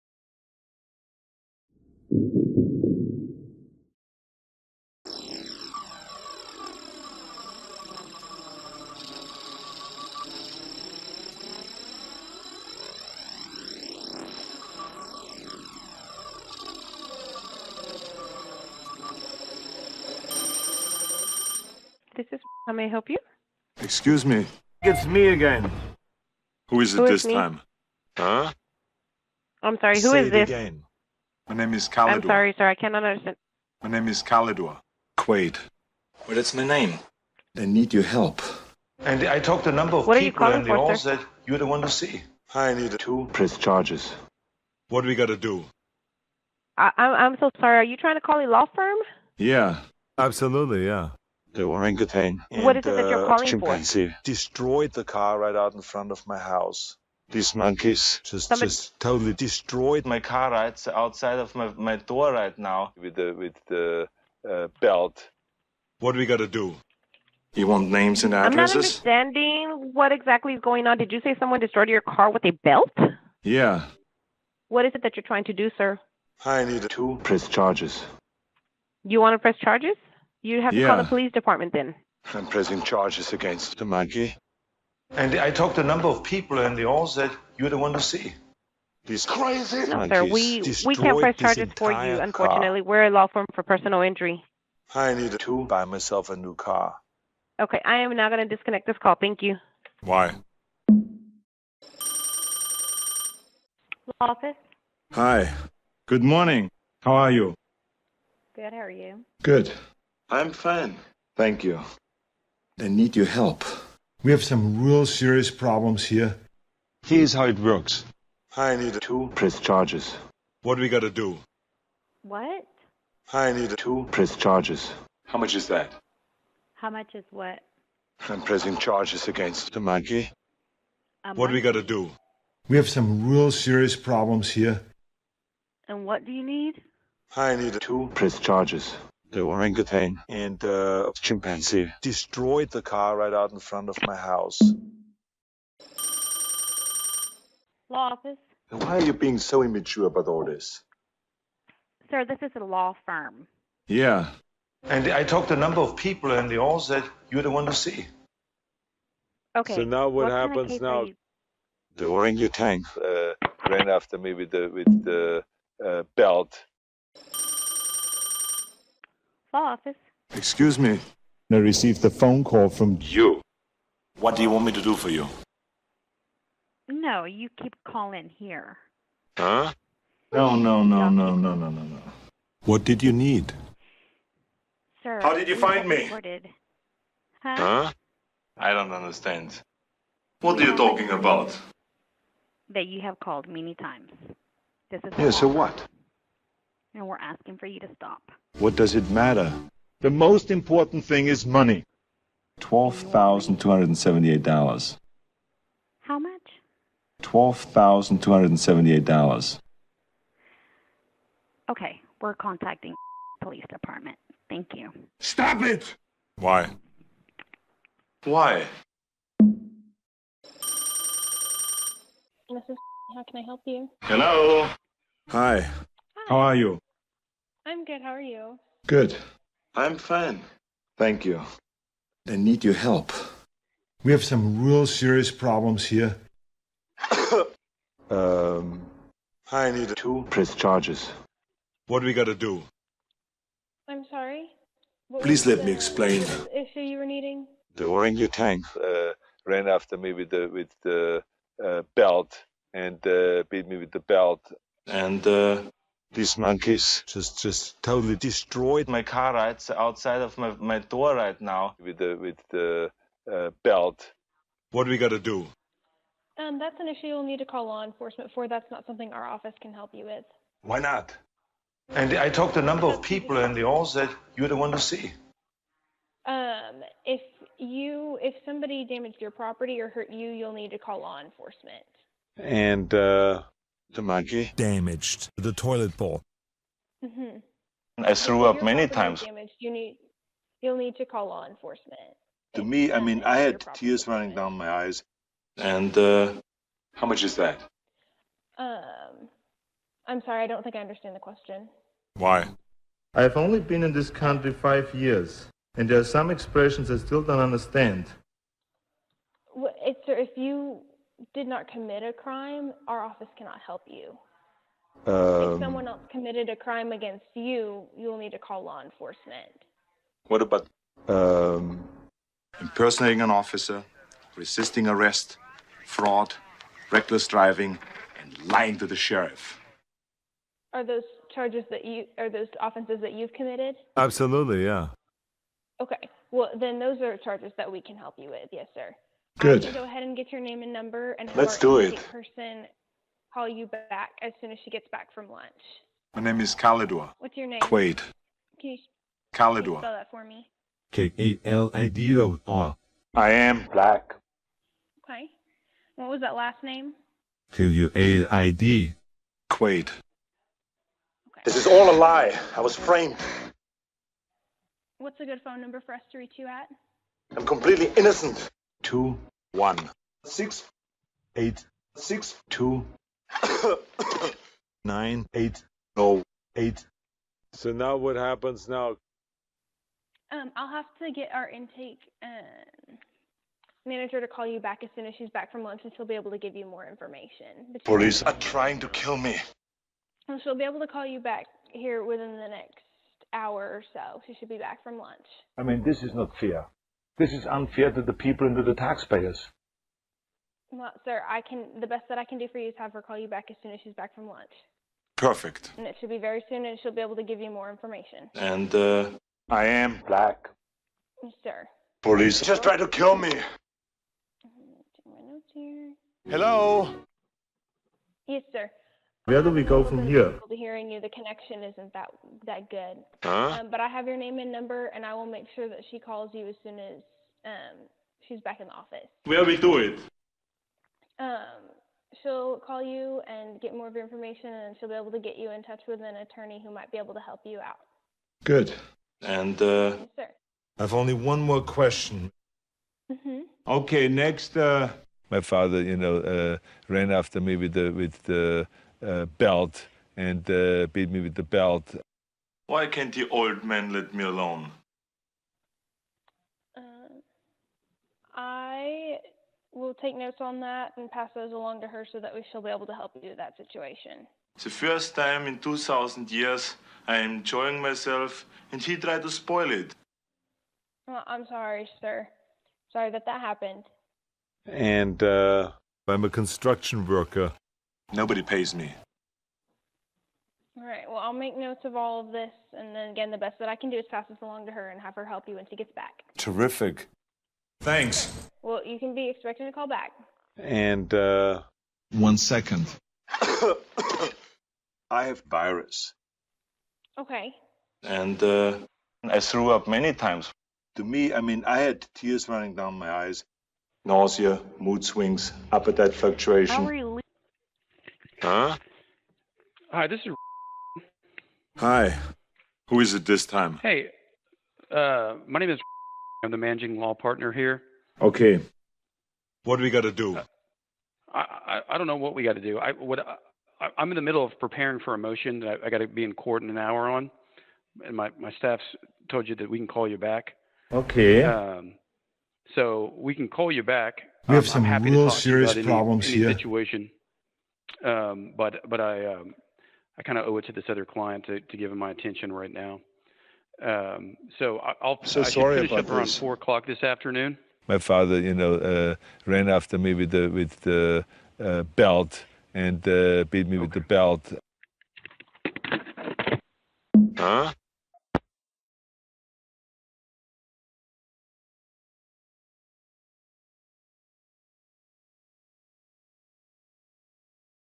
Arnold Terrorizes More Law Offices - Prank Call
Fighting the monkeys has proven fruitless, forcing Arnold to adjust his strategy by taking his war against the hairy miscreants to the legal front. With all the frivolous cases bouncing around America, surely there must be a lawyer out there willing to handle a serious case such as suing orangutans and chimps.

Call and soundboard created by AntiVenom9808. Special thanks to Realm of Darkness.

Soundboards used:
Arnold Schwarzenegger

Please leave any prank call or soundboard suggestions in the comments.